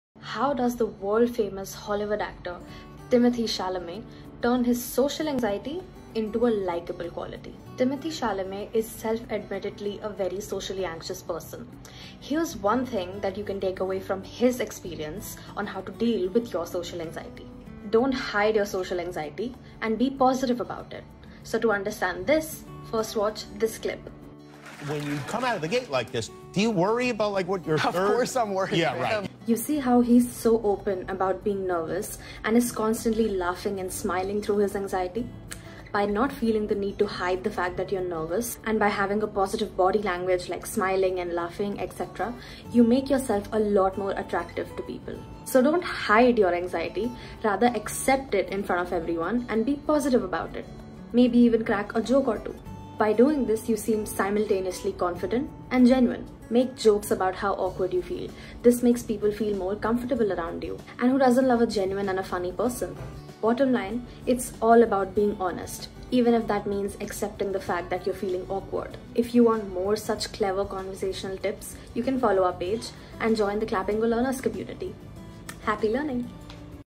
Timothee Chalamet Turns his Social Anxiety into a likeable quality
Bottom line- it’s all about being honest with yourself. And we are here to explain how😇

What also helps is to know that sometimes you can just simply listen and stay silent and not put so much pressure on yourself. 💯

If you have fun, everyone else has fun around you!

English Speaking ||  English Speaking with Clapingo || English Classes